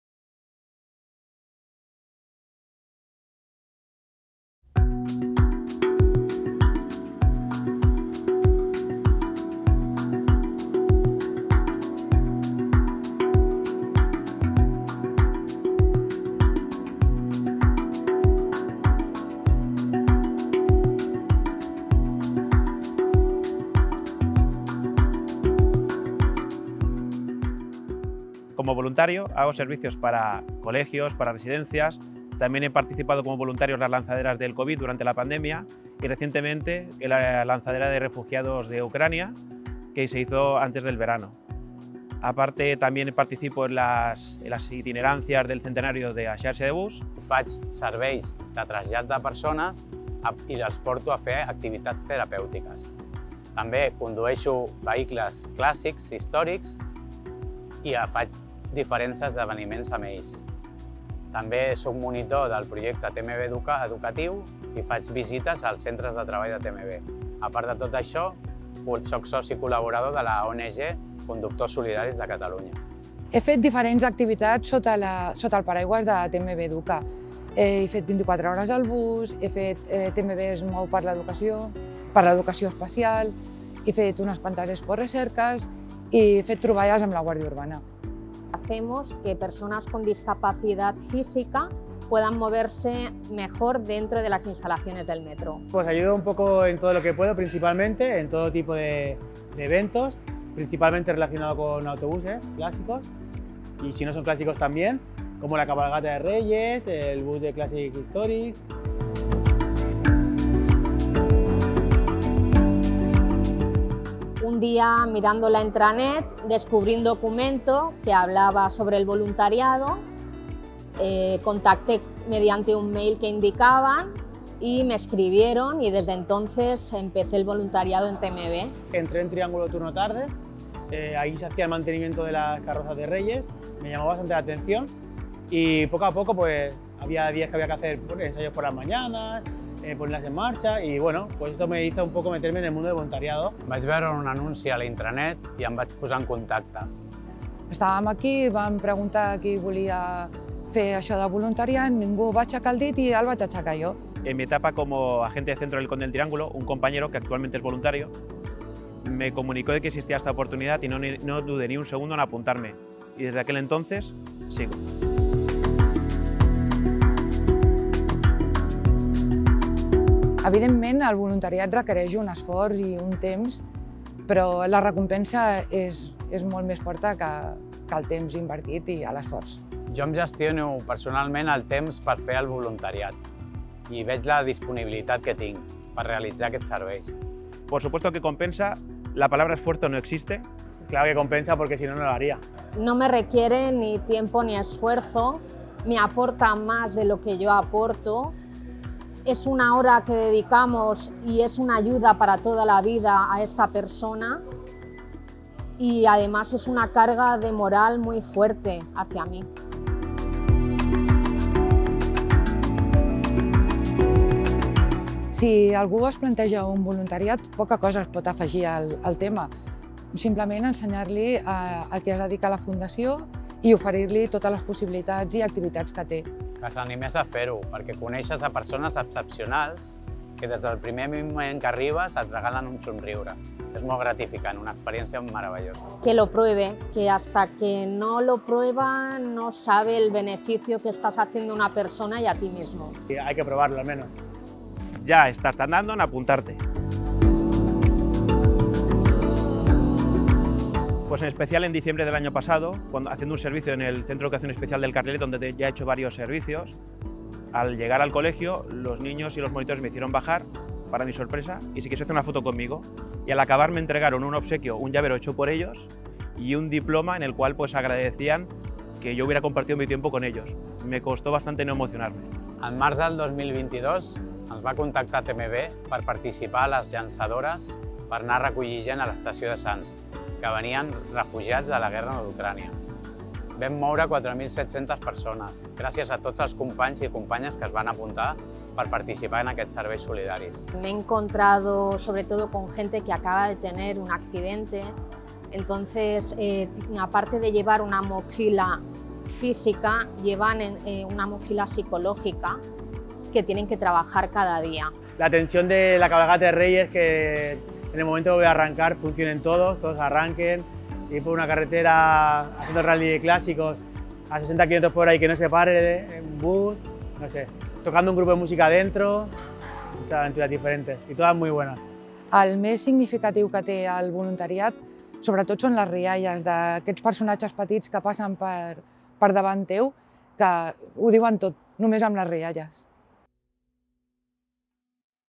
Voluntariat, un camí de doble recorregut: cinc experiències a TMB, en primera persona
Hi ha una opinió majoritària entre les persones que dediquen part del seu temps a ajudar els demès: reben més del que donen. Amb motiu del Dia Internacional del Voluntariat, cinc experiències personals a TMB ens animen a valorar sumar-nos-hi.